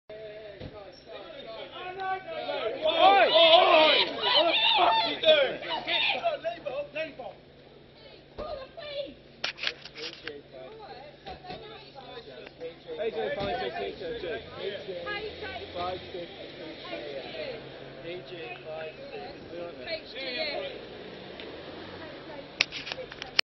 Charity ride road rage
Guildford cyclist Simon Page caught up in road rage incident